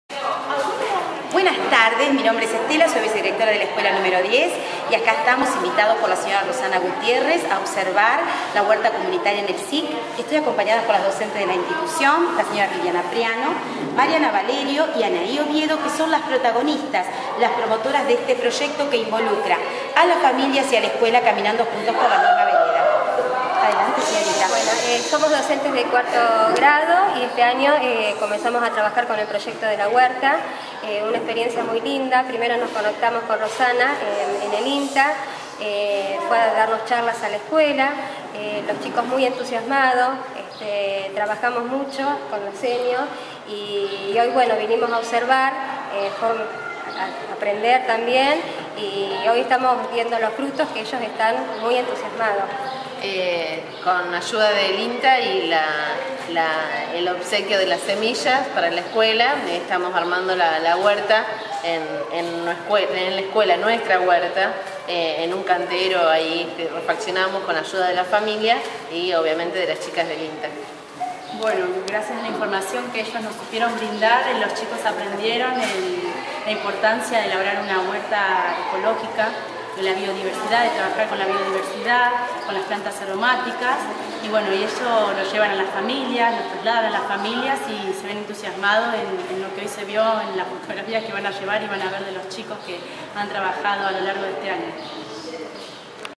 "La huerta bajo la lupa" - Escuela Nº 10 Zárate
Directivos y docentes de la Escuela Nº 10 de Zárate expresan su opinión acerca de la experiencia conjunta con el INTA para sus alumnos de 4º año.